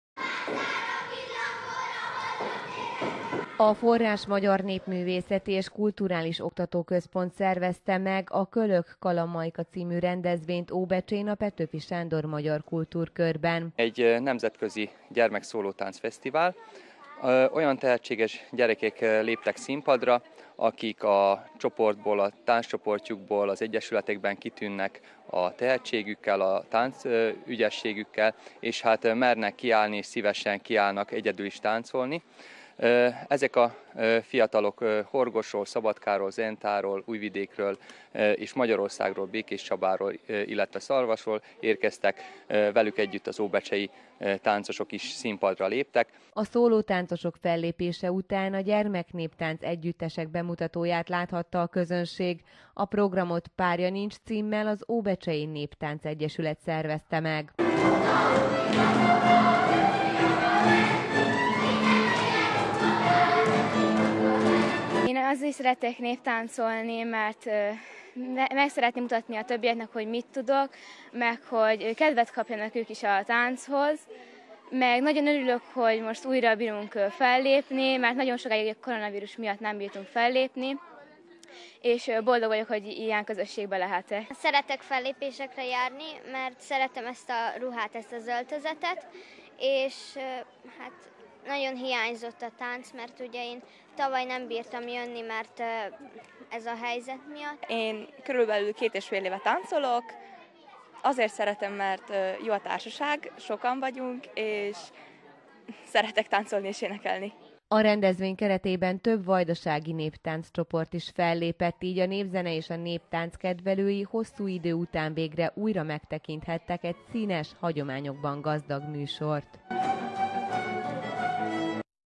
Néptánctudásukat mutatták be a fiatalok
A hétvégén két rendezvénnyel is készültek az óbecsei Petőfi Sándor Magyar Kultúrkör néptáncosai.